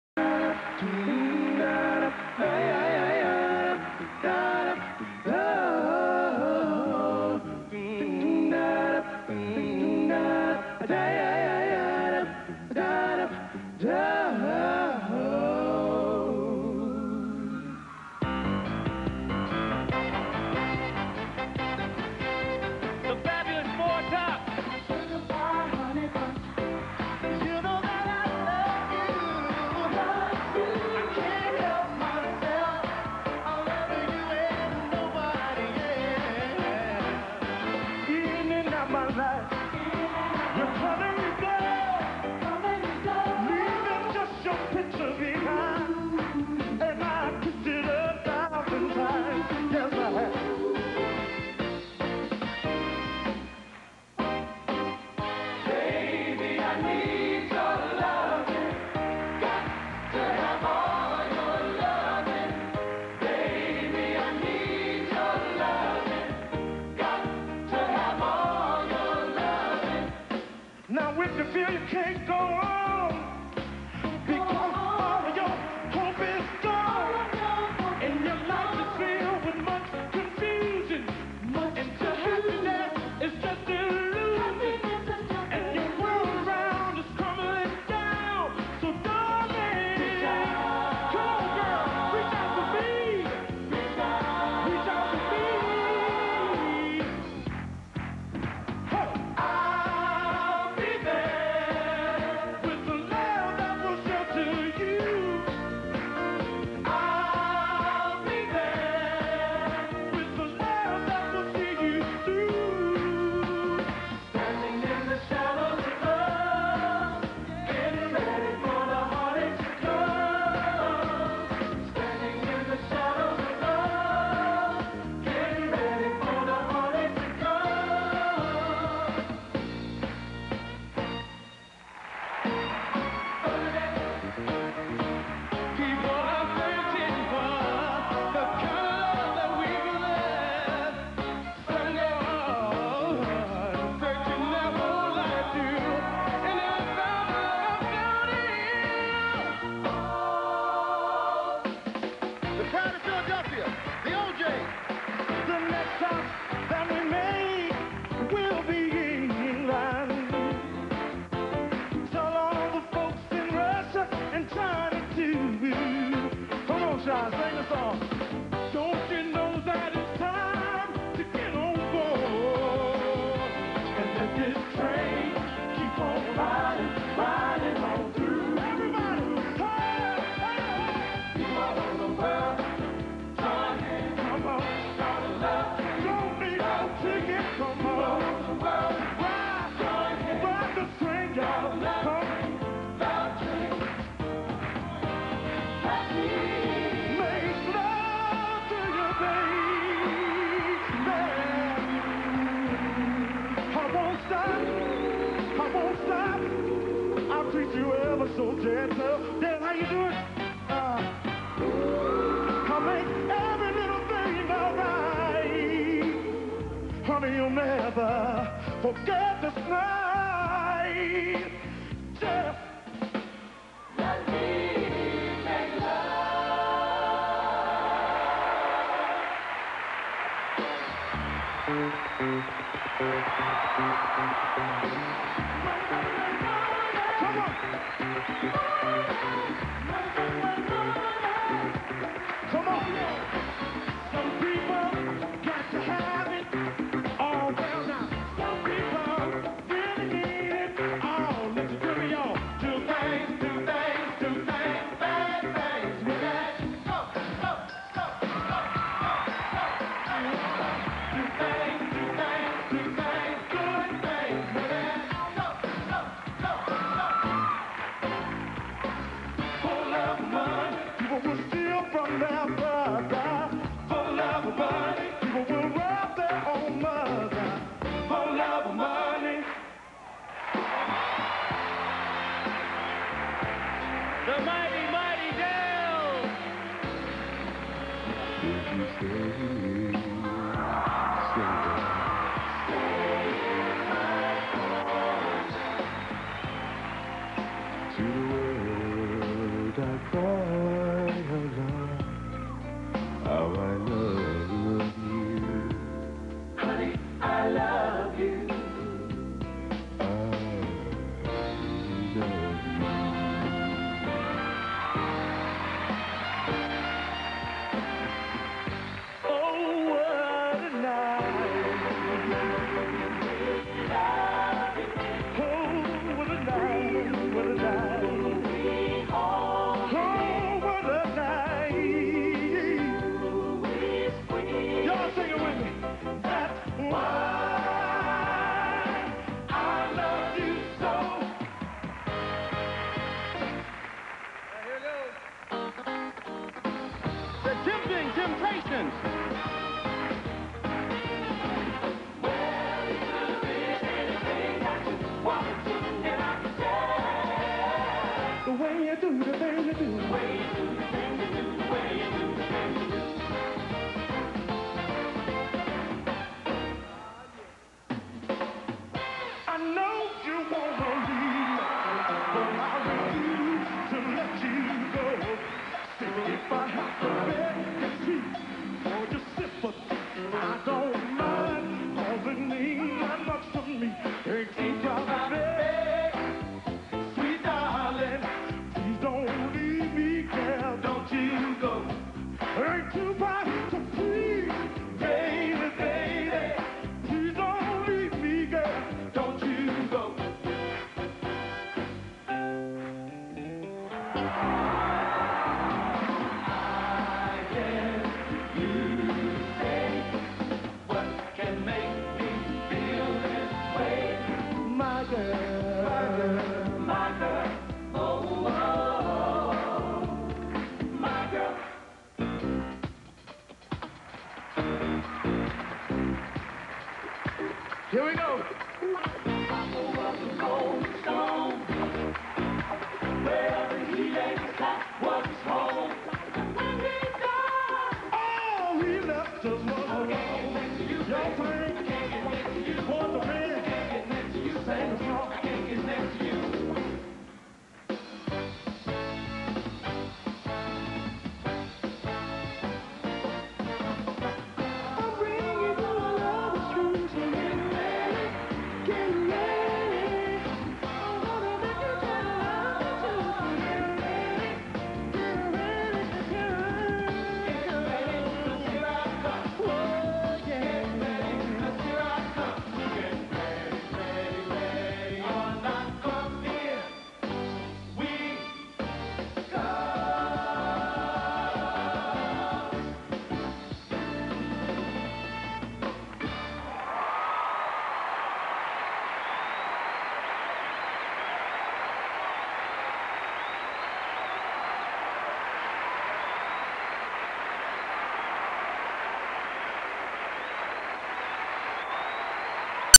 LEVERT AND BOYZ II MEN
LEVERT AND BOYZ II MEN PERFORM 60'S MEDLEY